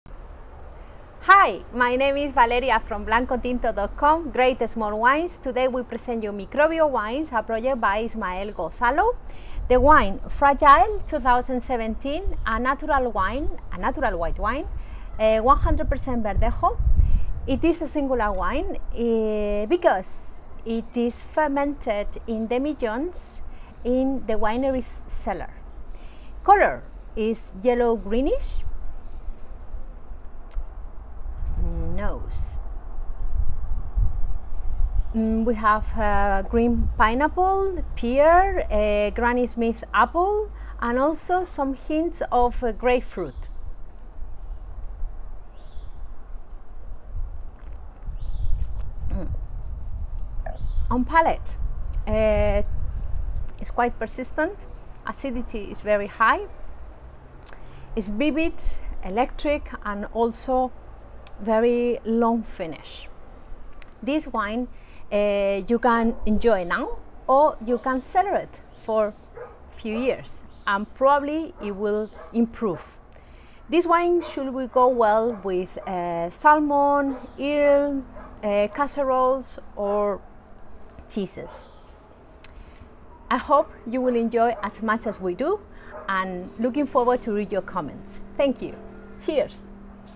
Wine Tasting | Castilla Leon Ismael Gozalo Microbio Wines Fragil 2017 white wine
This "Fragile" natural white wine made of 100% old vines verdejo is something special! It is fermented and aged in old glass demi-johns in the underground cellar. Less than 400 bottles per year... Don't miss this one!